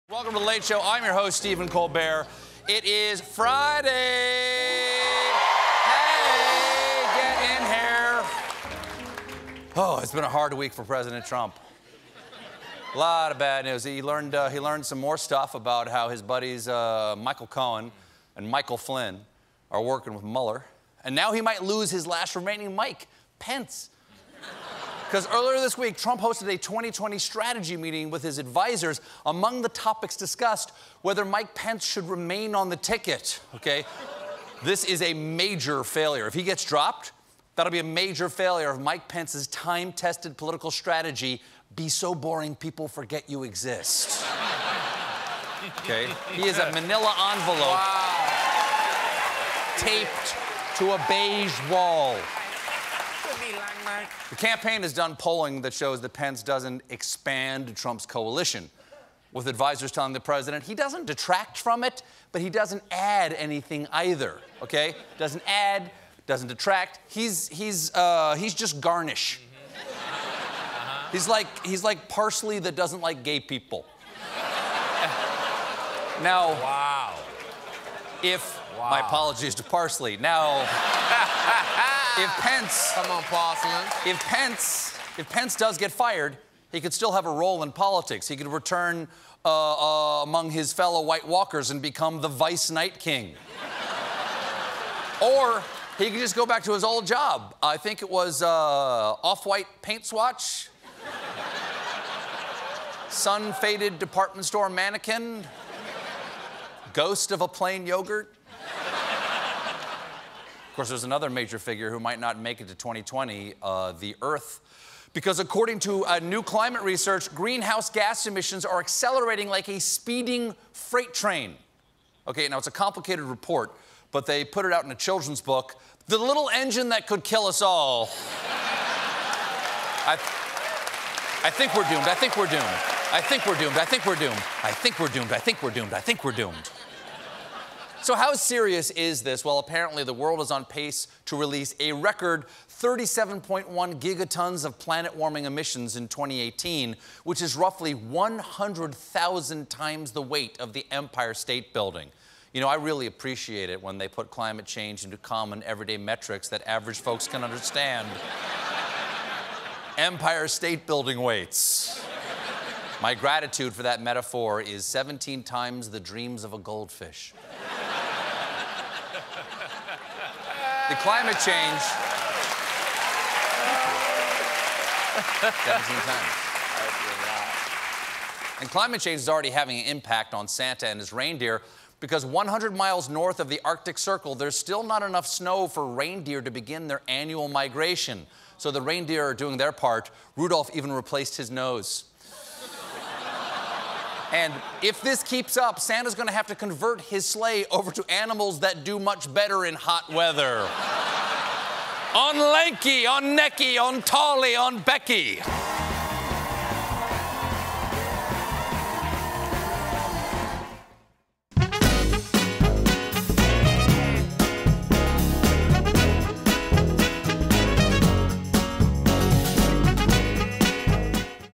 Mike Pence Isn't A Lock For The 2020 Ticket
Mike Pence was discussed at a recent 2020 strategy meeting between the President and his advisors. Which isn't a good thing for Mike Pence.